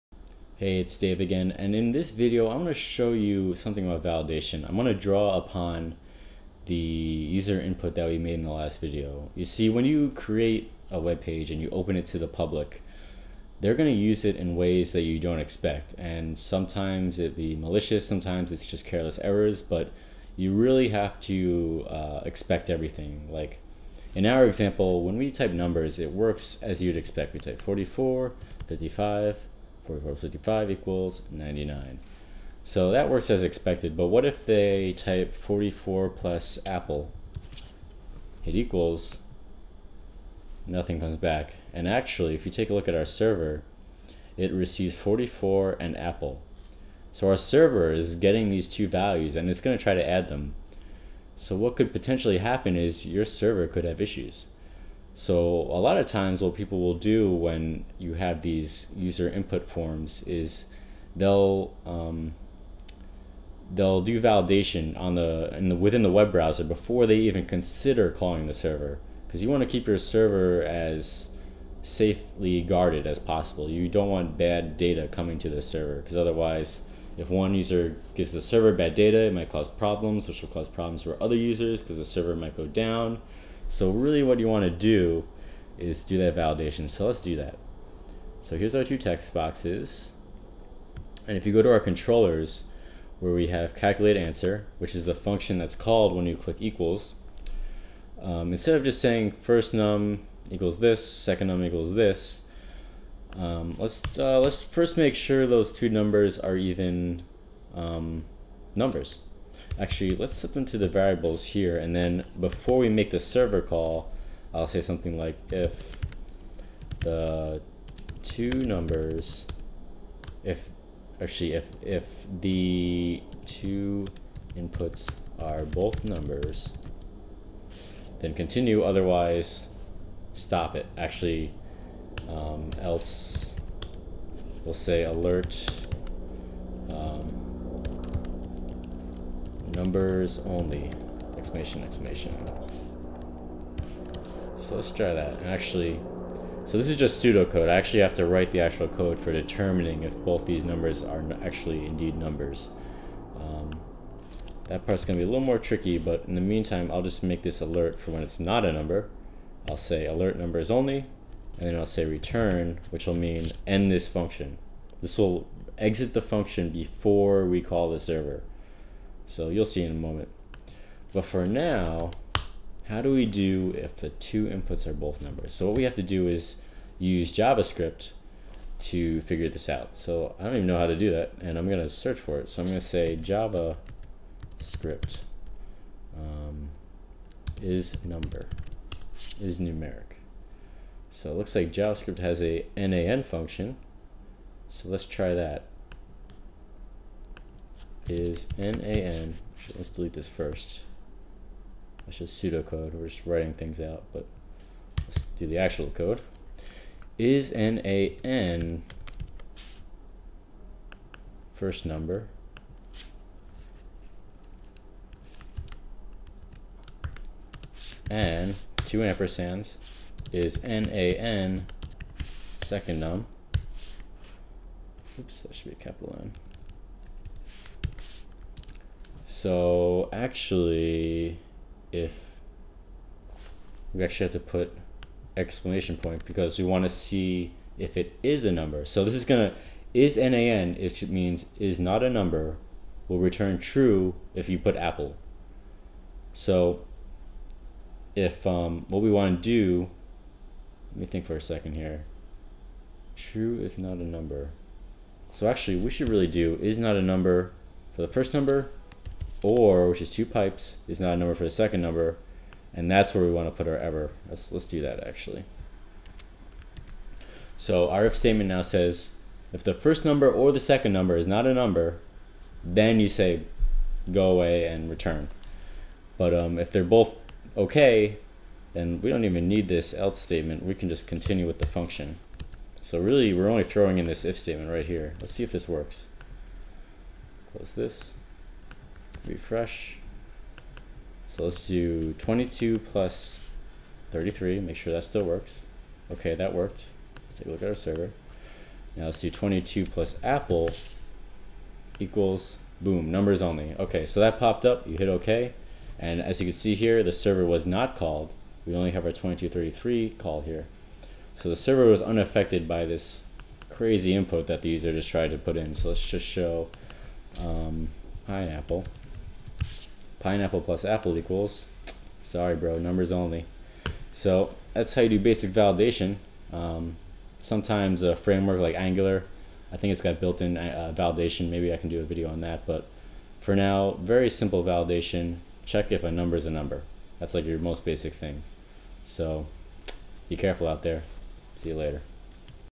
JavaScript #10: An Intro to User Input Validation
Here we just add in a quick if-statement right before our server call to make sure the data we send to the server is two numbers, and not something else!